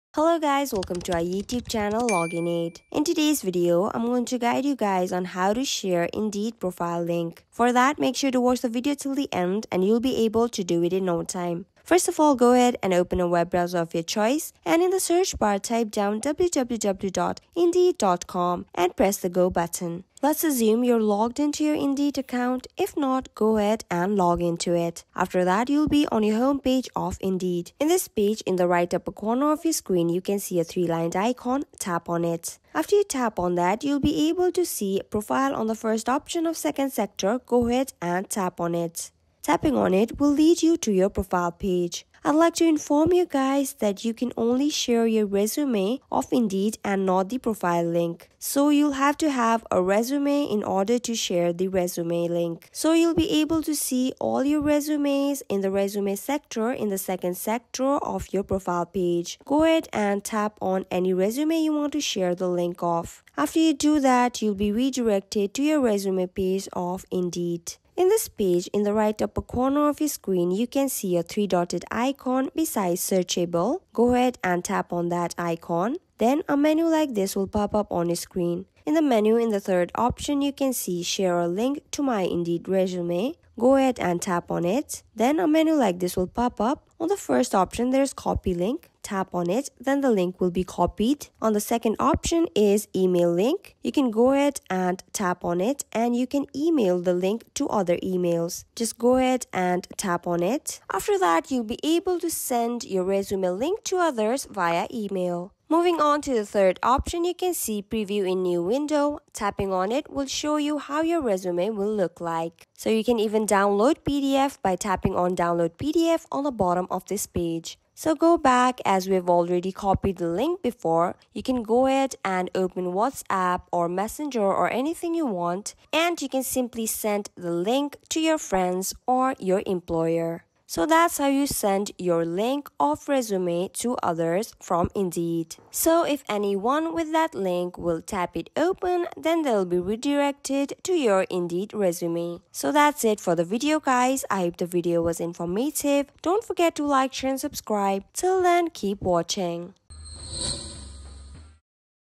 How To Share Indeed Profile Link 2024 | Copy/Send A Link To Your Indeed Resume
A video guide for the process to share/send Indeed profile link.